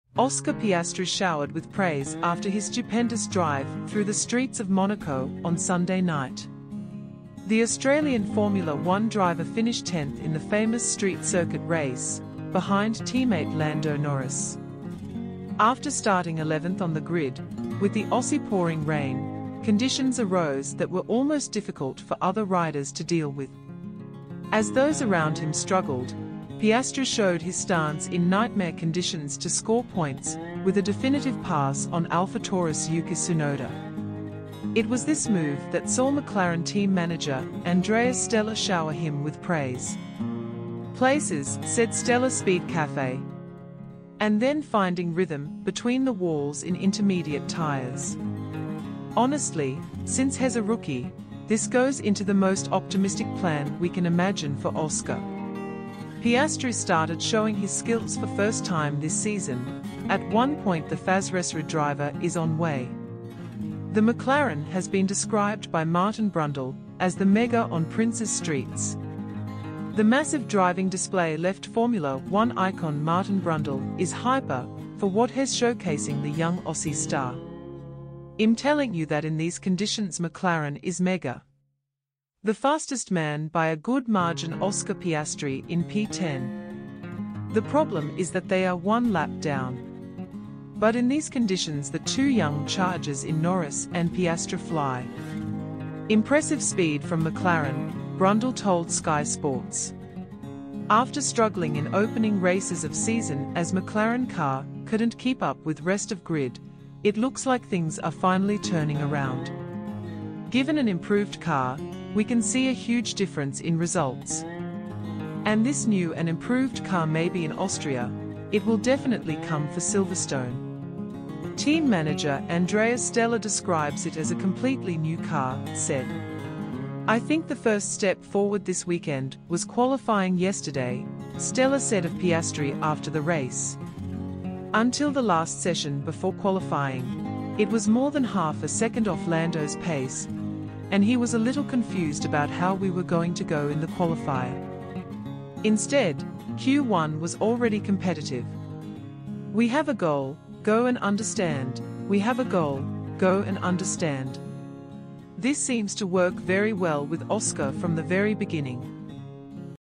F1 news 2023: McLaren boss praises ‘remarkable’ Oscar Piastri, Monaco Grand Prix, Lando Norris
Oscar Piastri showered with praise after his stupendous drive through the streets of Monaco on Sunday night. The Australian Formula 1 driver finished 10th in the famous Street Circuit race, behind teammate Lando Norris.  After starting 11th on the grid, with the Aussie pouring rain, conditions arose that were almost difficult for other riders to deal with. As those around him struggled, Piastri showed his stance in Nightmare Conditions to score points with a definitive pass on Alphatauri's Yuki Tsunoda. It was this move that saw McLaren Team manager Andreas Stella shower him with praise. places,” said Stella Speedcafe. “And then finding rhythm between the walls in intermediate tires. "Honestly, since he's a rookie, this goes into the most optimistic plan we can imagine for Oscar." Piastri started showing his skills for first time this season, at one point the Fasresr driver is on way. The McLaren has been described by Martin Brundle as the "Mega" on Prince's streets. The massive driving display left Formula 1 icon Martin Brundle is hyper for what he's showcasing the young Aussie star. “I'm telling you that in these conditions McLaren is mega. The fastest man by a good margin - Oscar Piastri in P10. The problem is that they are one lap down. But in these conditions the two young charges in Norris and Piastri fly. Impressive speed from McLaren ,” Brundle told Sky Sports. After struggling in opening races of season as McLaren car couldn't keep up with rest of grid, it looks like things are finally turning around. “Given an improved car, we can see a huge difference in results. And this new and improved car maybe in Austria, it will definitely come for Silverstone. Team manager Andreas Stella describes it as a completely new car,” said. “I think the first step forward this weekend was qualifying yesterday,” Stella said of Piastri after the race. “Until the last session before qualifying, it was more than half a second off Lando's pace and he was a little confused about how we were going to go in the qualifier. “Instead, Q1 was already competitive. “We have a goal, go and understand; we have a goal, go and understand. ” “This seems to work very well with Oscar from the very beginning.”